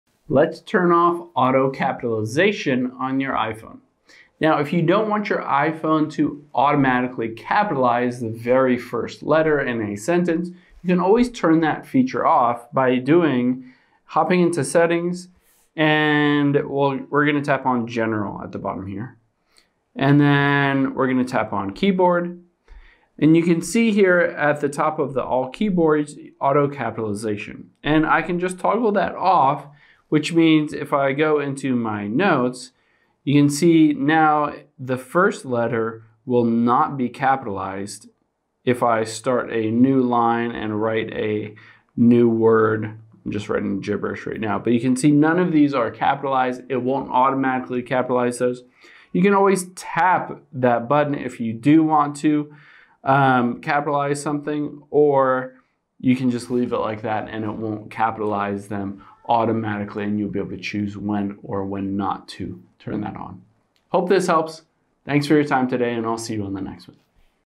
How To Turn Off Auto Capitalization On iPhone
Let's disable auto caps on your iPhone if you prefer the keyboard to not automatically capitalize the first letter of a sentence.